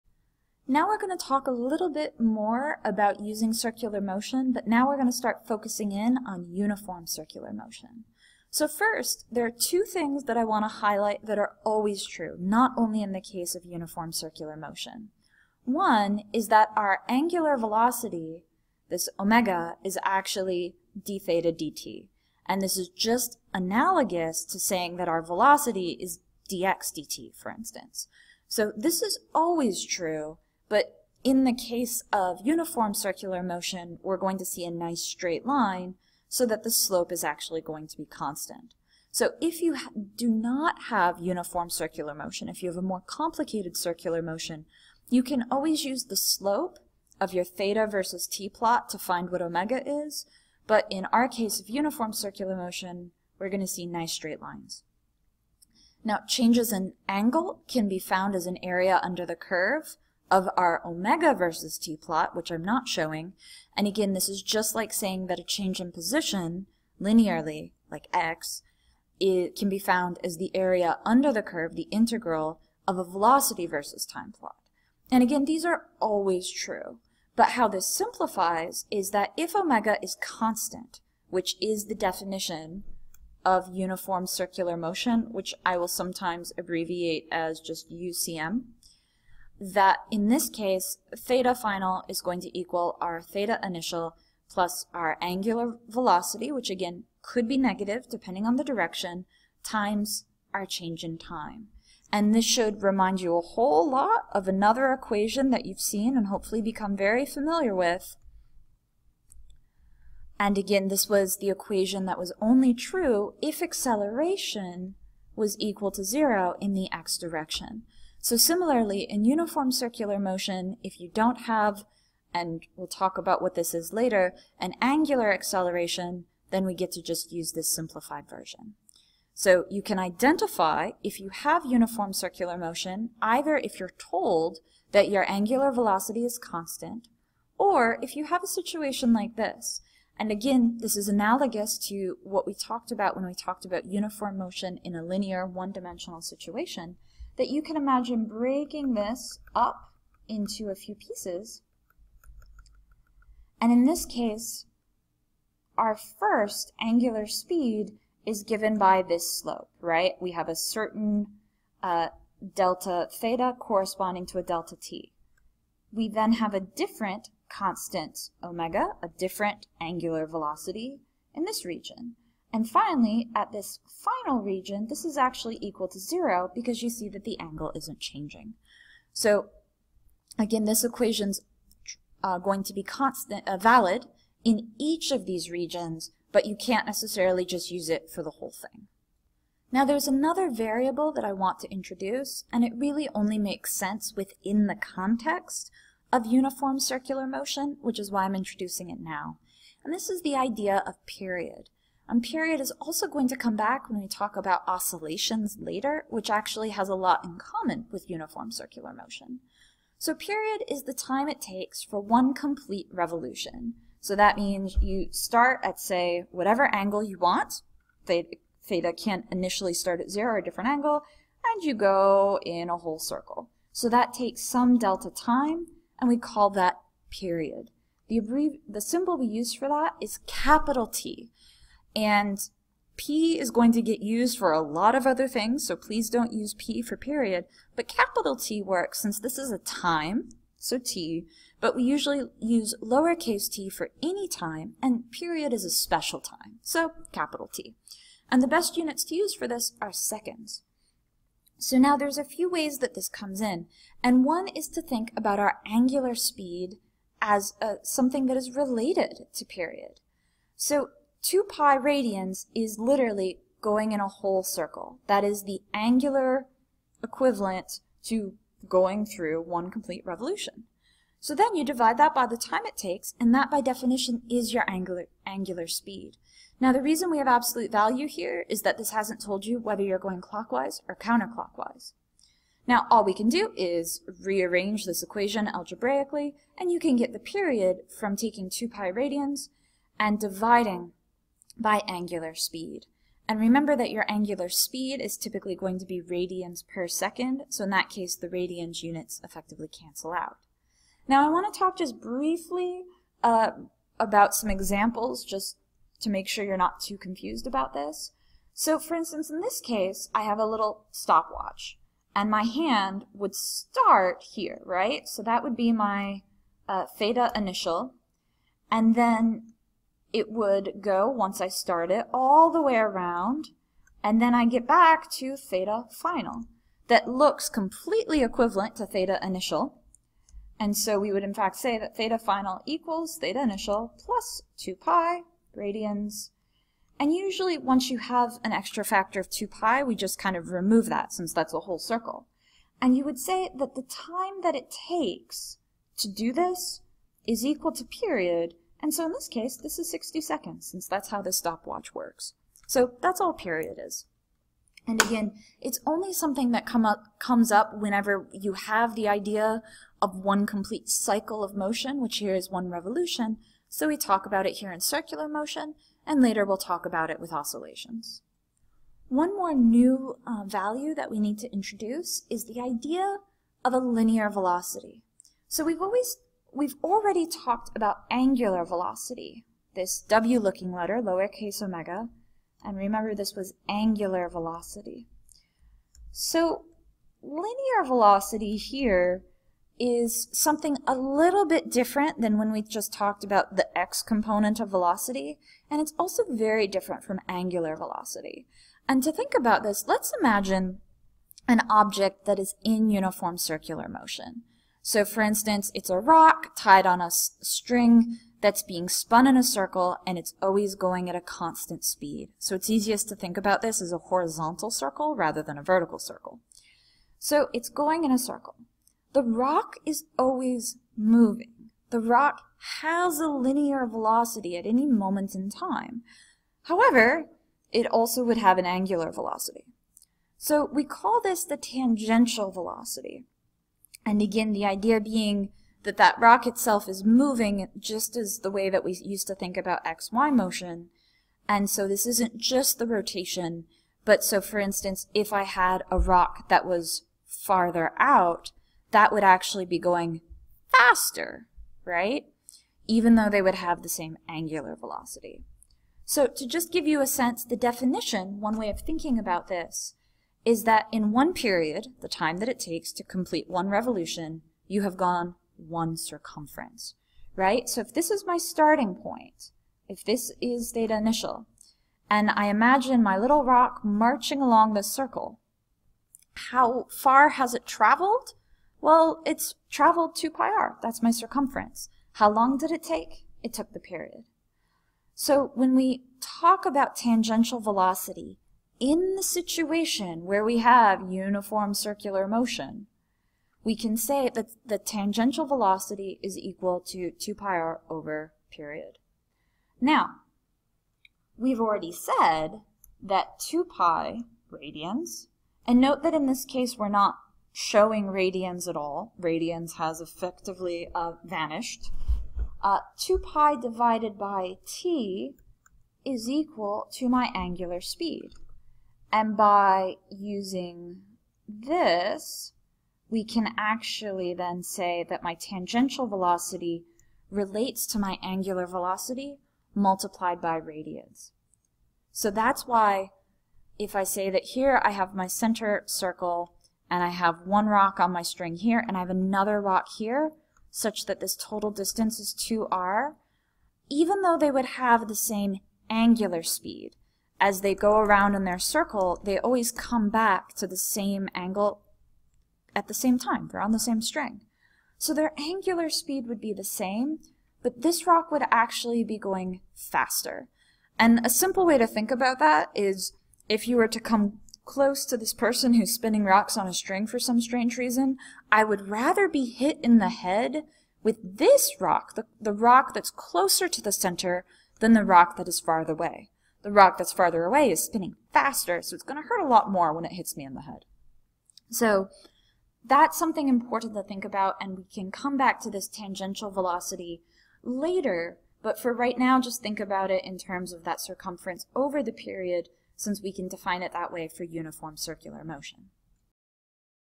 Circular Motion: Uniform Circular Motion
Physics 202 at Agnes Scott College.  Week 3.  Circular Motion - Uniform Circular Motion, Period, tangential speed.